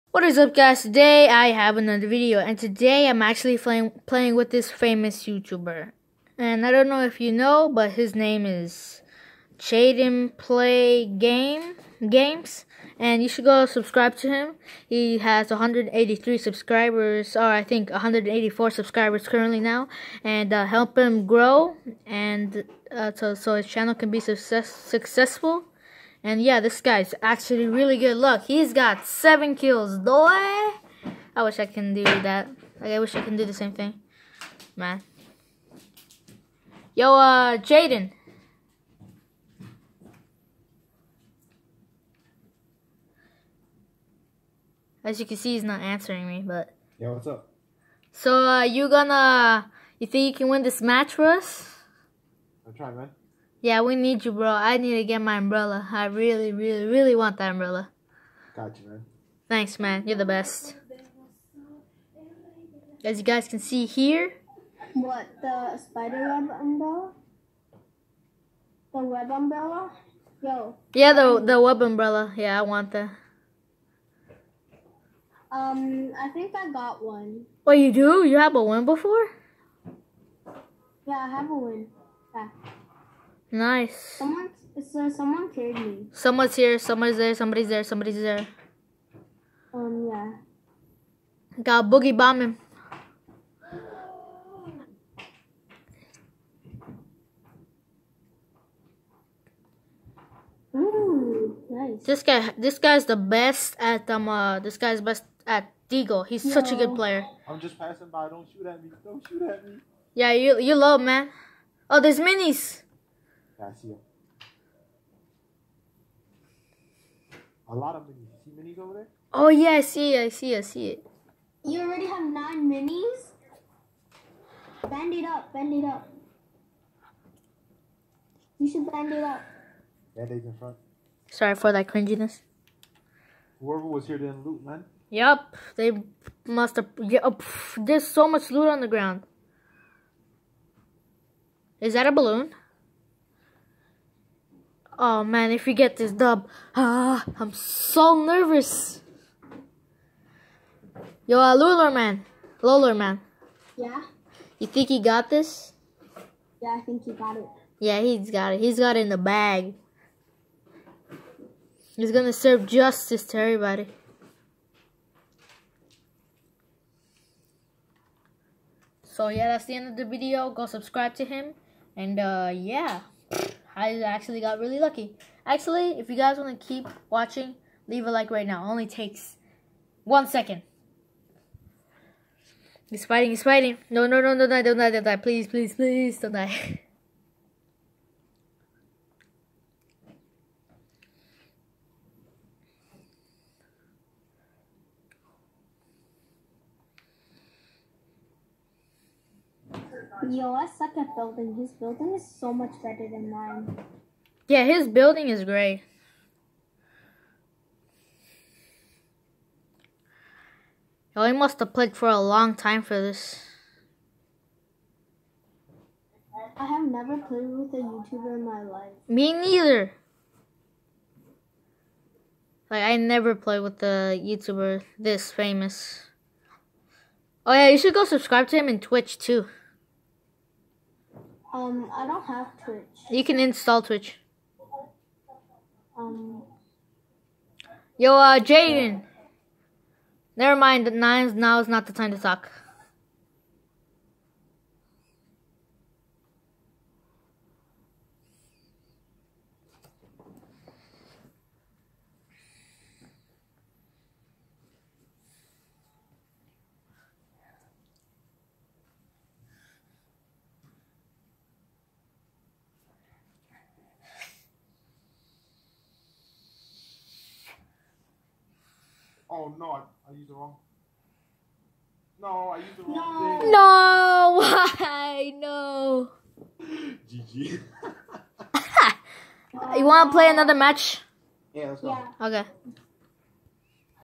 This is amazing I so lucky. Read description
So yeah I was playing random squads when I met this famous guy. He's YouTube channel name is Jaden play games. And like always stay AWESOME.